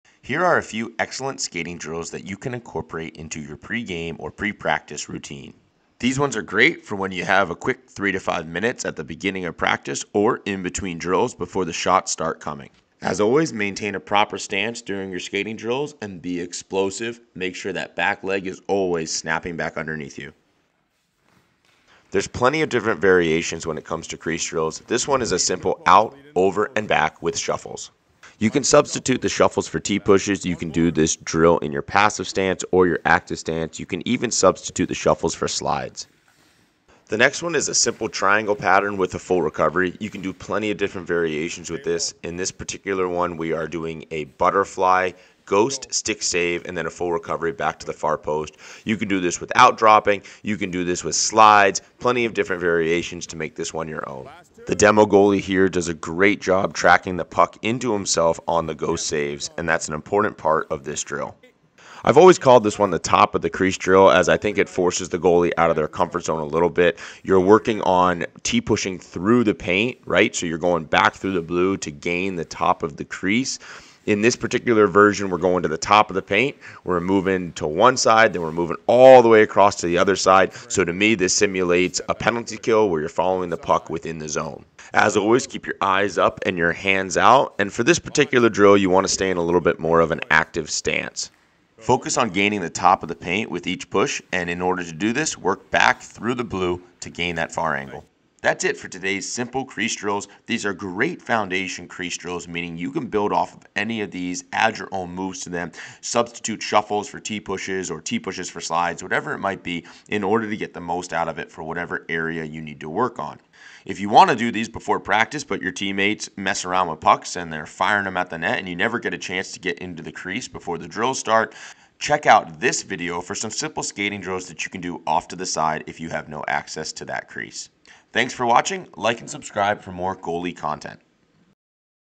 Goalie Crease Movement Warm Up Drills | Hockey Goalie Tips and Drills
Goalies: We know that team practices are never geared toward goalie development, so we have to ask ourselves, what is within our control?

Use the crease drills in this video as foundation for warm up skating pre-practice and pre-game.

Goalies: Be creative but structured in your skating warm ups. Focus on fundamental movements and visualizing saves.

The drills in this video are malleable, meaning you can substitute t-pushes for slides, shuffles for full recoveries etc.

Regulation pucks
Shot With →
Phone Holder
Lightweight TriPod
S22 Ultra
AKASO Camera